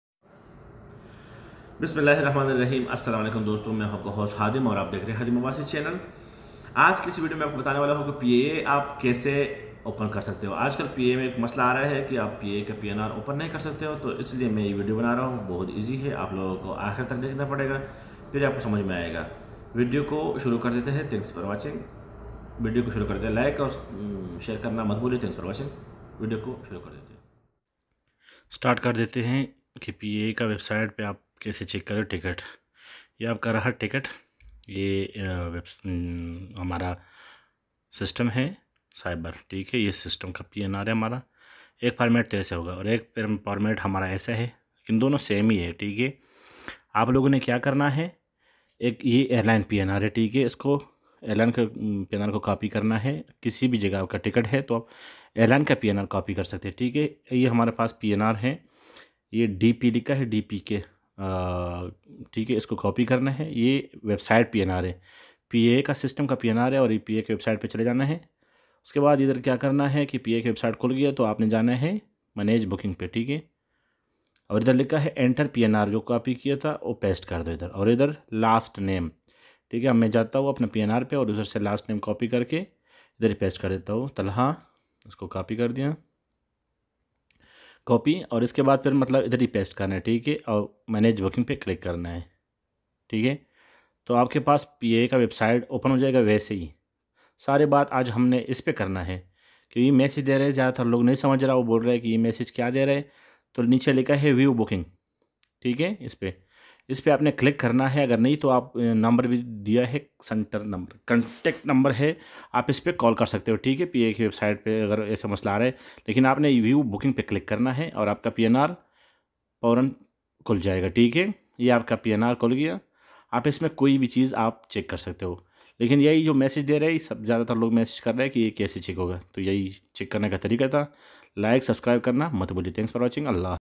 How to check PIA New System and PIA Problem Solve By Khadim Abbasi
پی آئی آے کا نیا سسٹم چیک کرنا اس ویڈیو میں چیک کر سکتے ہے۔
اور سسٹم بھی آسان ہے بس اس ویڈیو آخر تک دیکھو
شکریہ